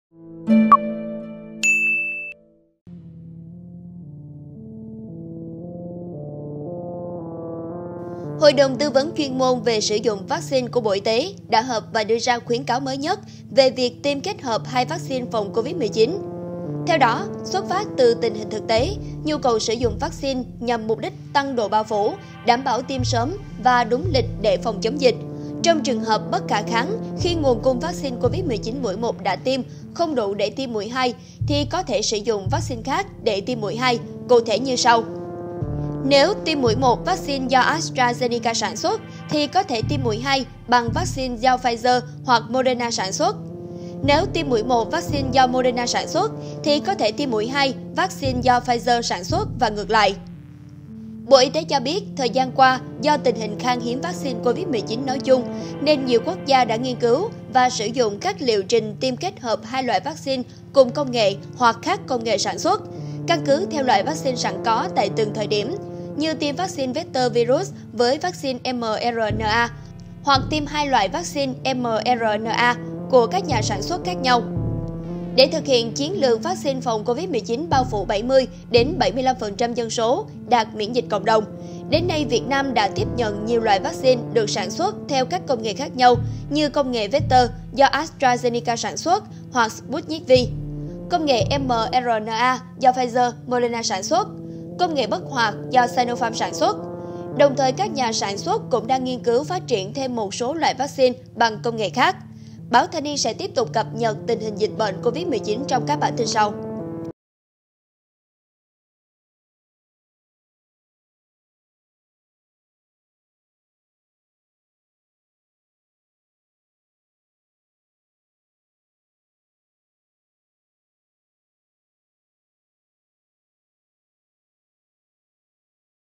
Khuyến cáo mới nhất về tiêm kết hợp 2 vắc xin Covid-19
Hội đồng tư vấn chuyên môn về sử dụng vắc xin của Bộ Y tế đã họp và đưa ra khuyến cáo mới nhất về việc tiêm kết hợp 2 vắc xin phòng Covid-19.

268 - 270 Nguyễn Đình Chiểu, Phường Võ Thị Sáu, Q.3, TP HCM